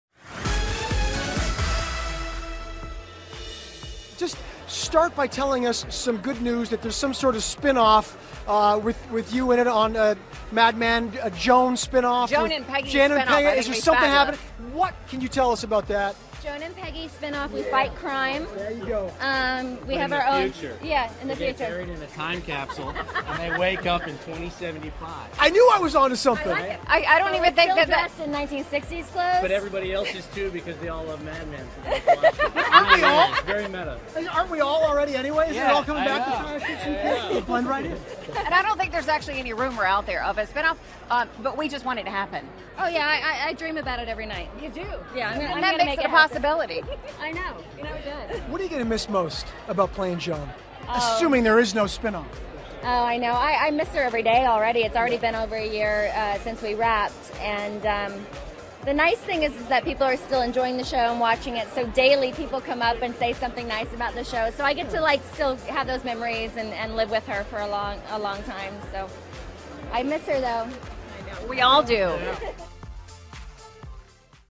EXCLUSIVE: Christina Hendricks Dreams of a 'Mad Men' Spin-Off Every Night
The star teases ET with her idea for a show about Joan and Peggy -- in the future!